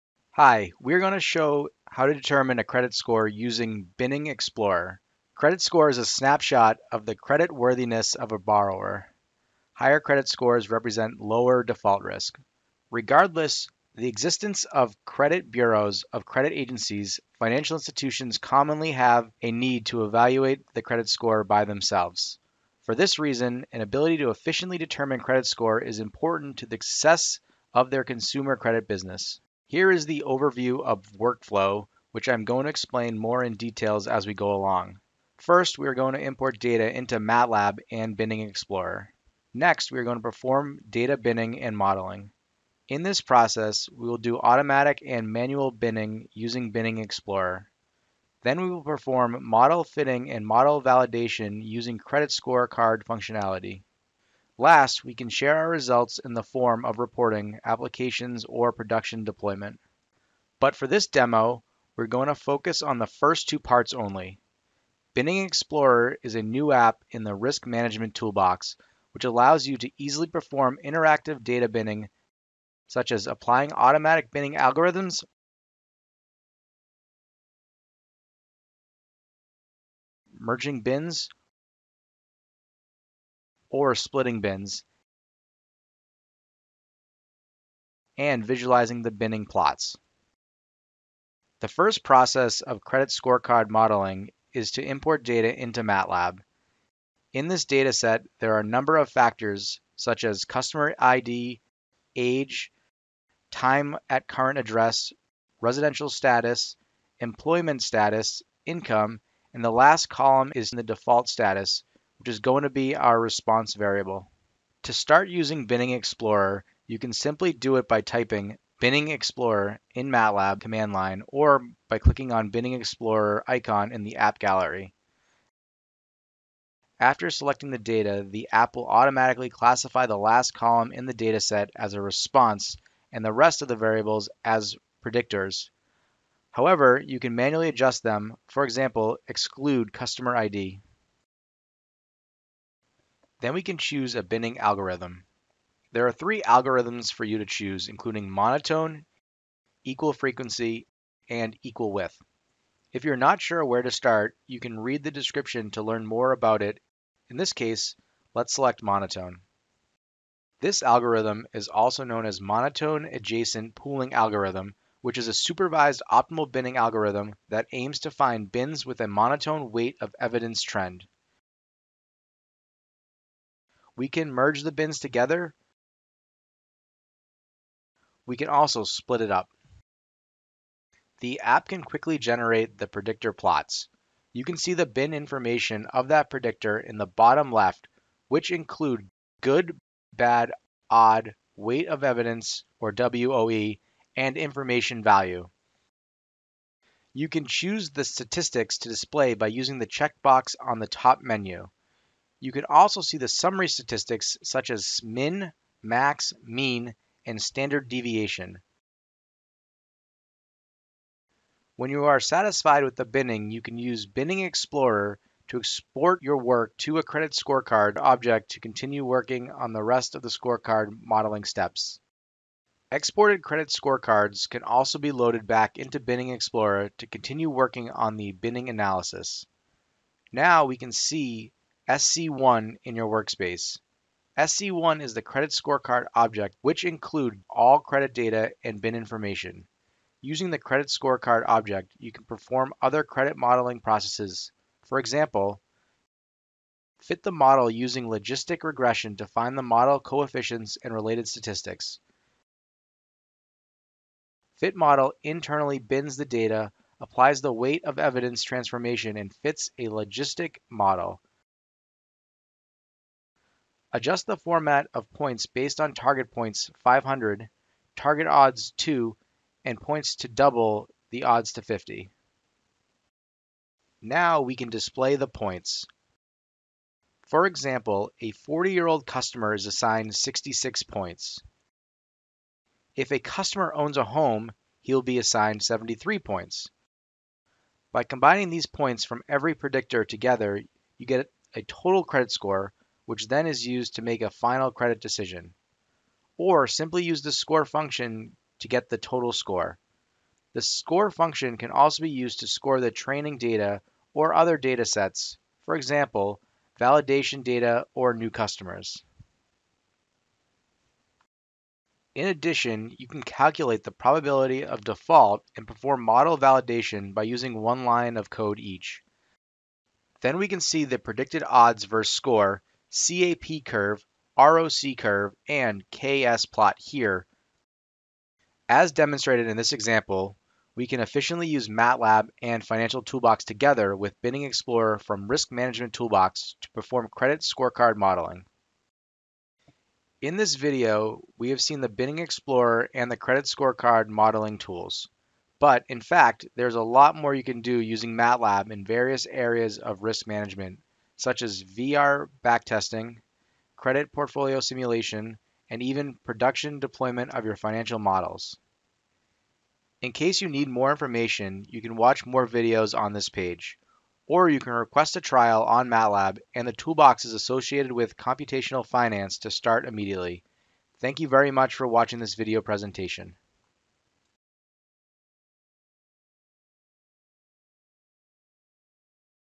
Credit Scorecard Modeling Using the Binning Explorer App - MATLAB Video
Use the Binning Explorer app, a new app in Risk Management Toolbox™ to perform data binning and modeling. Use the built-in functions in MATLAB® together with Financial Toolbox™ to perform other processes in credit score modeling, allowing you to analyze consumer credit risk in an efficient manner.